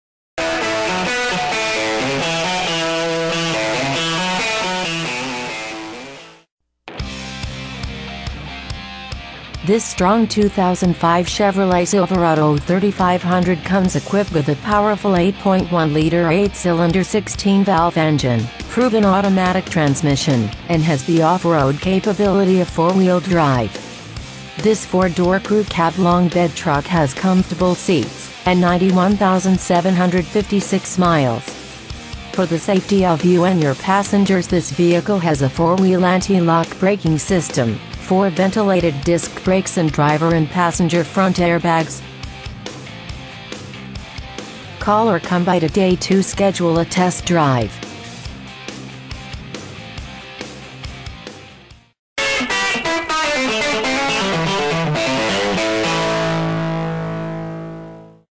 2005 Chevrolet Silverado 3500 Spring TX 77388
Year: 2005
 Make: Chevrolet
 Model: Silverado 3500
 Engine: 8.1 liter 8 cylinder 16 valve
 Trans.: Automatic
 Exterior: Summit White
 Miles: 91,756